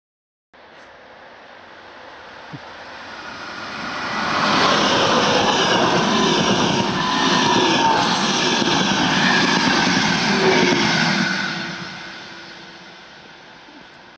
Železnice a sníh.
Rychlík z Prahy před stanicí Hluboká - Zámostí.
Leden 2023.